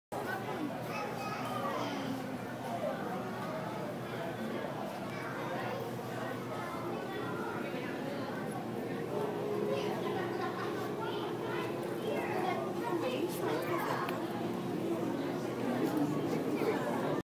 Teenie Tiny Jellyfish
More Jellyfish.  Taken on my iPhone 4.  Very dark lighting conditions.